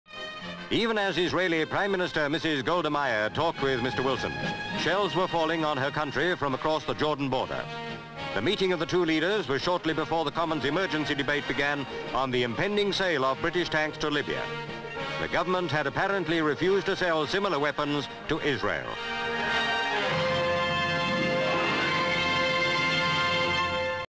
Mrs Meir At No 10 Aka Israeli Pm Sees Mr Wilson (1969)
No title.  Israeli Prime Minister, Mrs. Golda Meir, meets Prime Minister Harold Wilson at No. 10 Downing Street, London.

Various shots Downing Street.  M/S crowds outside No. 10.  Interior shot showing Mrs. Meir and Mr. Wilson walking towards fireplace.  C/U Wilson and Golda Meir, pan down to them shaking hands.  Panning shot Wilson and Golda Meir walking away from fireplace to go into another room.
 FILM ID:2211.12